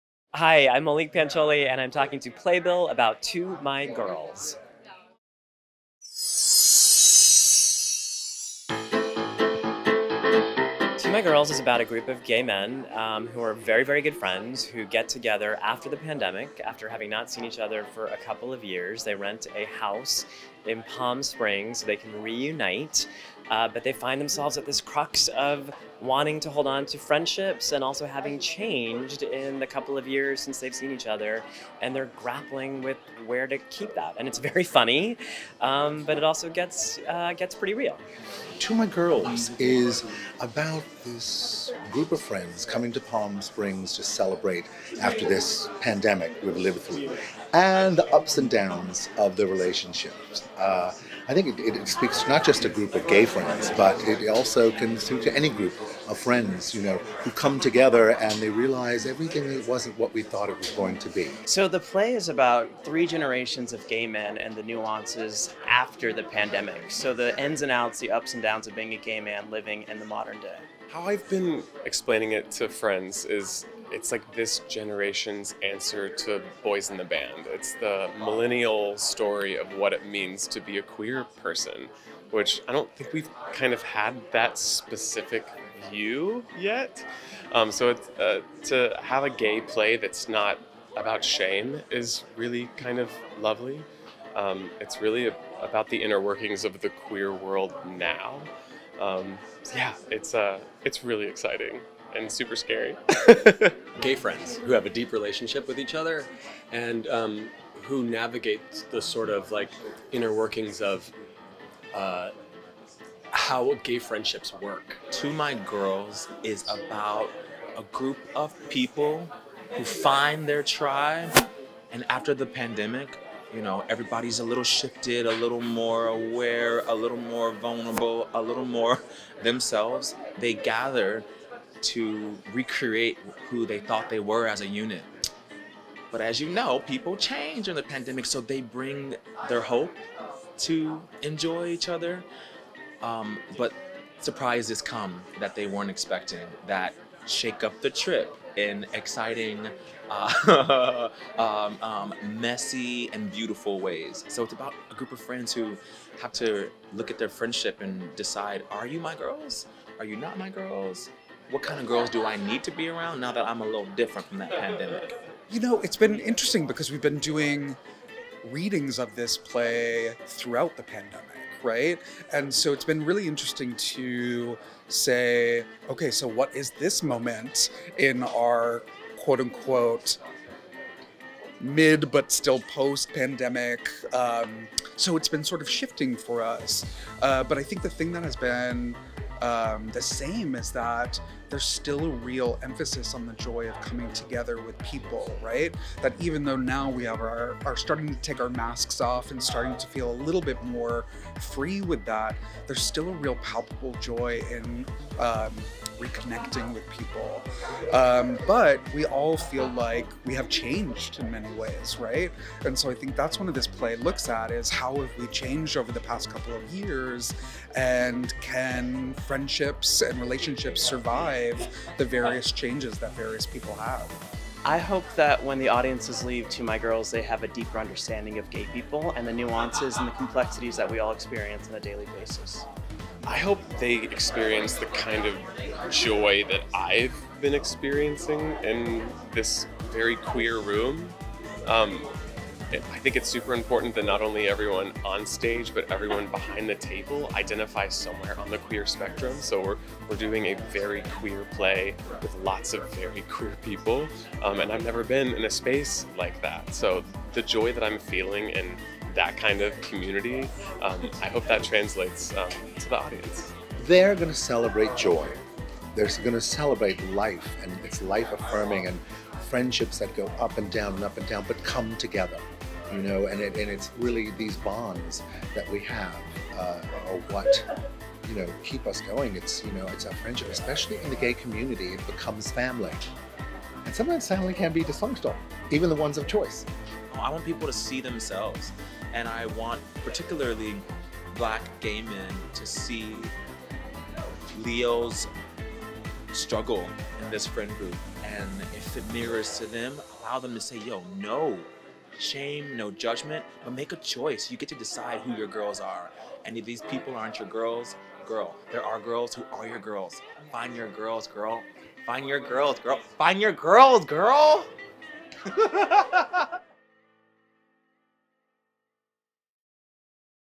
Jay Armstrong Johnson, Maulik Pancholy, and More On What to Expect From JC Lee’s To My Girls
To My Girls, a new play by JC Lee, begins performances March 15 at Second Stage’s Tony Kiser Theater. The play centers on a friend group of gay men enjoying a post-pandemic vacation in Palm Springs who, after the drinks start pouring, discover some hard truths about a changed world. Watch the video above to learn more about what to expect.

The production stars Bryan Batt (Mad Men, Jeffrey), Jay Armstrong Johnson (On the Town, The Phantom of the Opera), Carman Lacivita (Marvin's Room, Cyrano de Bergerac), Maulik Pancholy (30 Rock, It's Only a Play), Noah J. Ricketts (Frozen, Beautiful), and Britton Smith (Be More Chill, Shuffle Along).

A Strange Loop and Be More Chill director Stephen Brackett will helm the production, leading a creative team that includes scenic designer Arnulfo Maldonado, costume designer Sarafina Bush, lighting designer Jen Schriever, and sound designer Sinan Refik Zafar. Movement is by Patrick McCollum, and casting is by The Telsey Office.

To My Girls is a Donna and Ben M. Rosen commission, and the production is supported in part by an award from the National Endowment for the Arts.